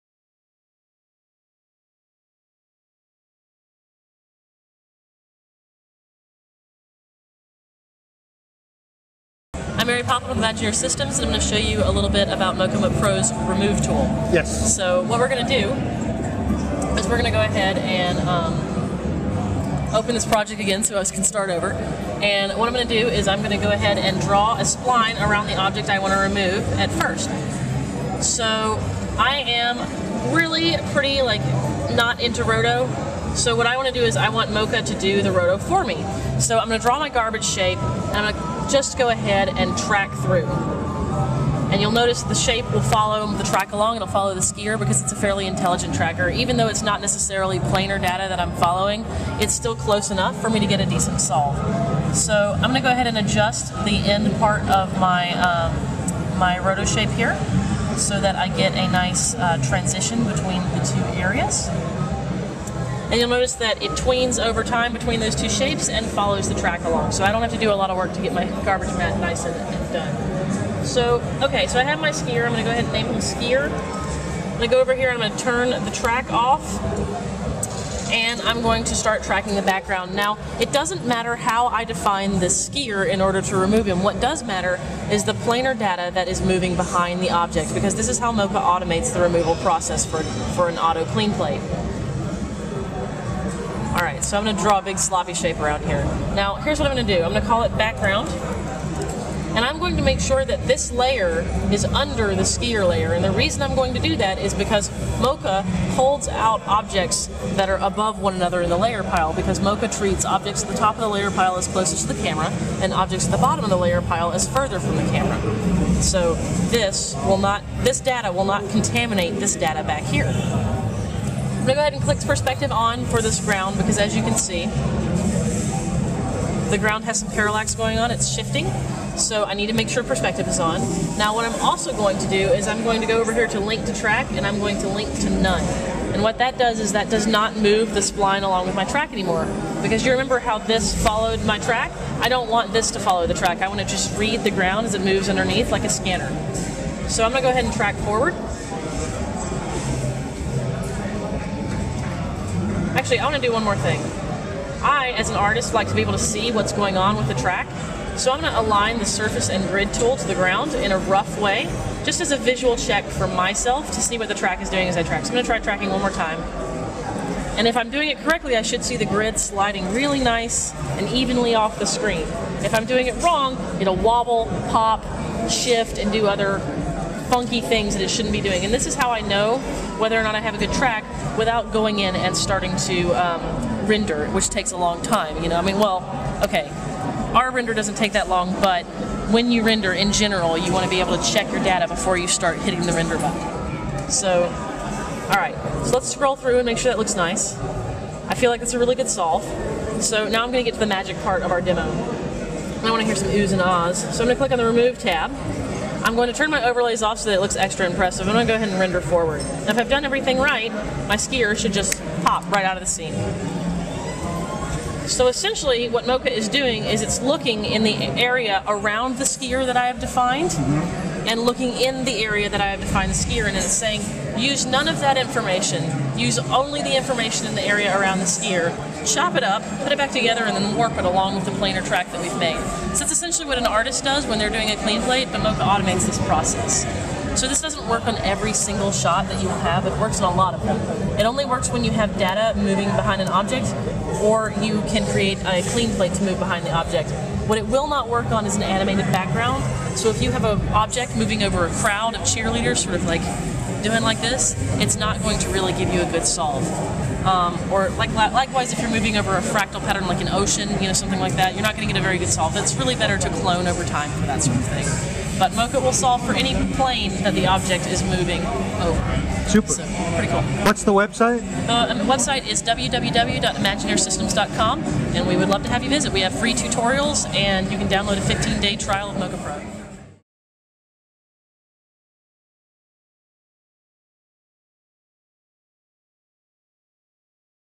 mocha Pro remove tool demo at Siggraph 2011
Mary Poplin, product specialist, demonstrates the "Remove Tool" which is part of the planar tracking program mocha Pro developed by Imagineer Systems. This video was shot at the IS booth during Siggraph 2011 in Vancouver, B.C., Canada.